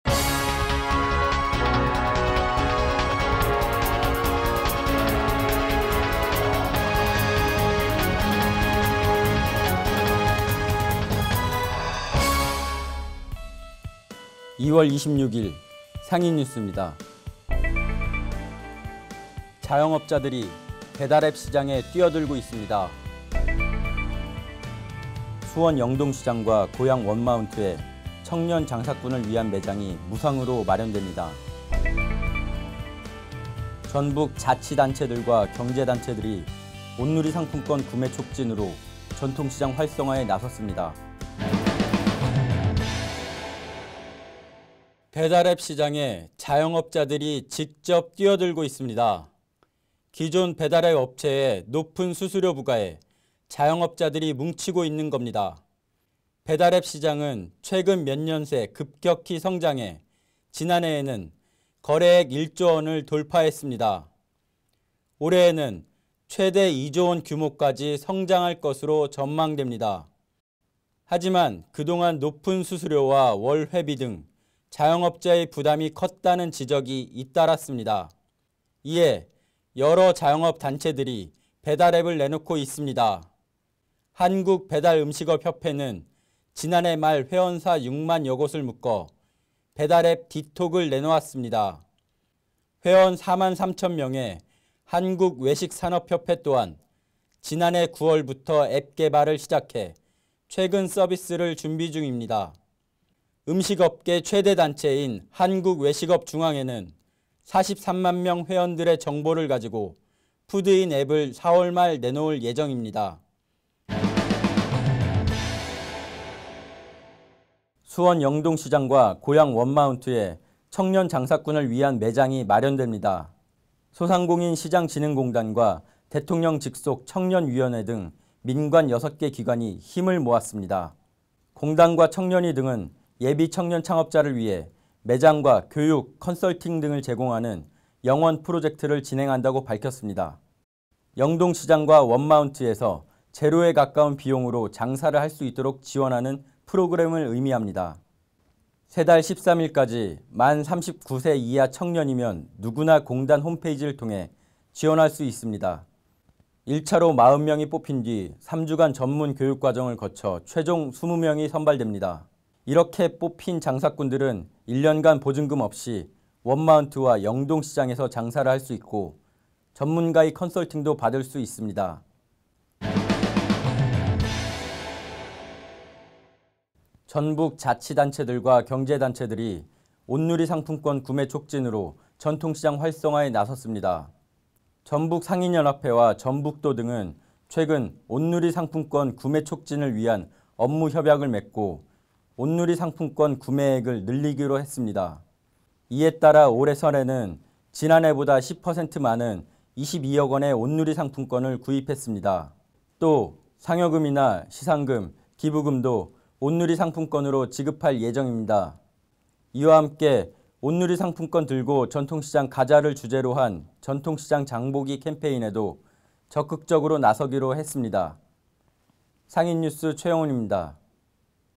자영업자, 배달앱 직접 만든다 외, 상인뉴스 19회
본 영상은 중소벤처기업부의 예산 지원을 받아
소상공인 및 전통시장 활성화를 위한 공익적 목적이 담긴 영상임을 알려드립니다.
*** 추가로 본 영상은 2015년에 제작된 영상입니다***

1. 자영업자들, 배달앱 직접 만든다
2. 청년장사꾼에 장소 무상지원…‘영+원 프로젝트’
3. 전북 자치·경제단체…전통시장 활성화 발벗고 나서